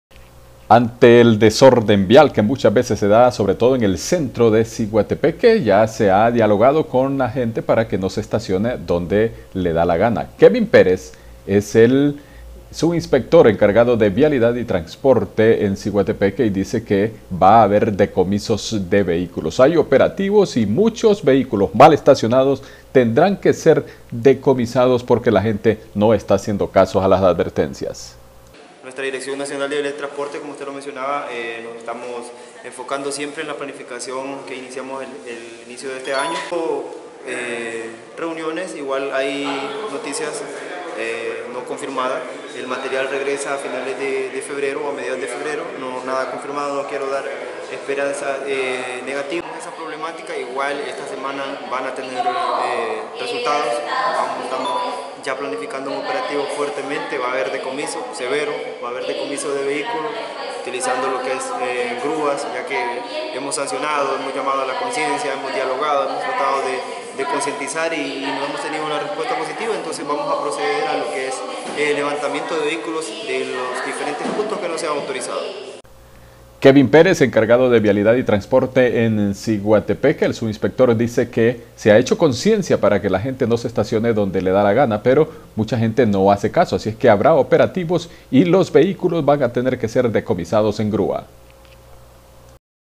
Se usará grua para levantar vehiculos mal estacionados
EL sub inspector Kevin Pérez lamenta que se estacionen mal , pero si sigue esa conducta, se van a llevar con grua a todos los autos mal estacionados, esto en Siguatepeque.